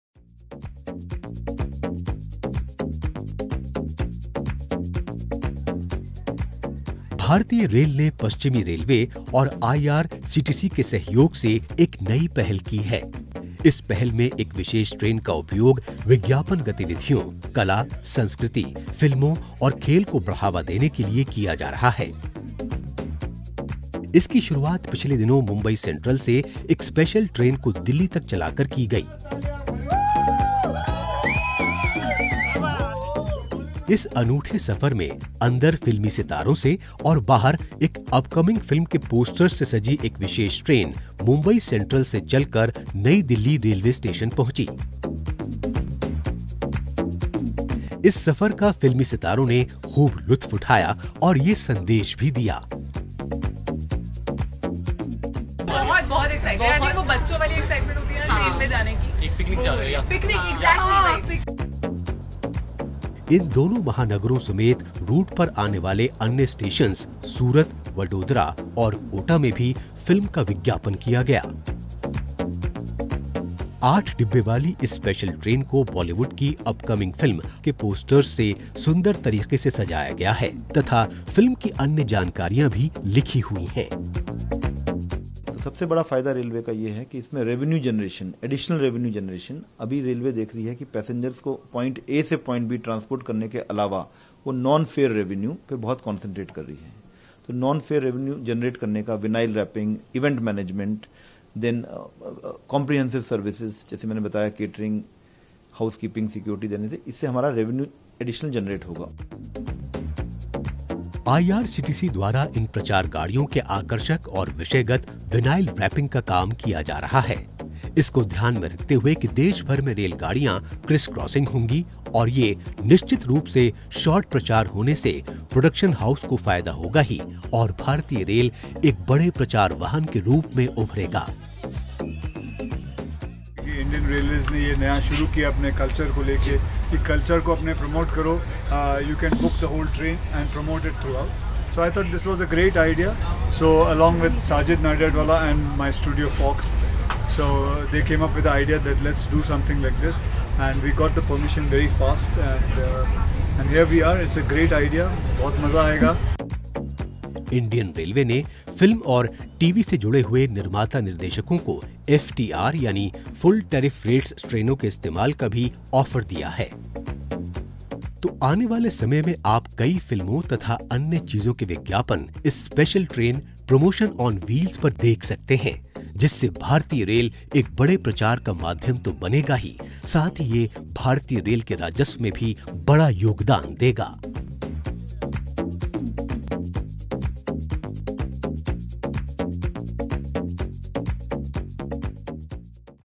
कला, संस्कृति, फ़िल्मों एवं खेलकूद आदि के प्रोत्साहन हेतु विशेष ट्रेन ‘प्रमोशन ऑन व्हील्स’ चलाई गई
भारतीय रेल द्वारा कला, संस्कृति, फ़िल्मों,टी वी एवं खेलकूद आदि के प्रोत्साहन हेतु  विशेष ट्रेन ‘प्रमोशन ऑन व्हील्स’ चलाई गई है। इससे रेलवे को अतिरिक्त राजस्व की भी प्राप्ति हुई ।